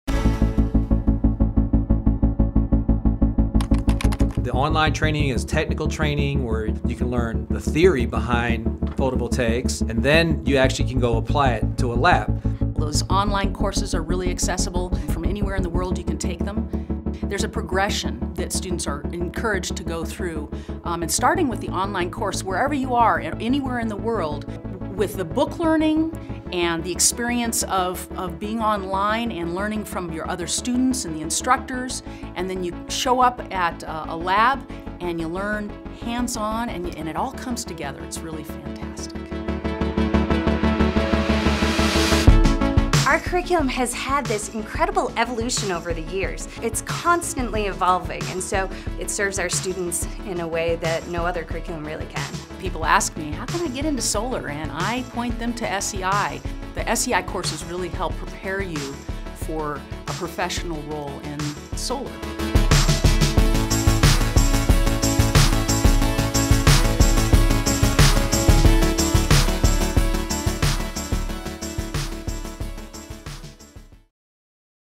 SEI ONLINE CAMPUS OVERVIEW
SEI's Online Campus is the best way for busy solar professionals and people wanting to get into the solar career field to take their technical training.  With over 75,000 alumni worldwide - the SEI Online Campus allows our non-profit school to reach thousands of people every year creating the world's largest online learning community in the renewable energy sector.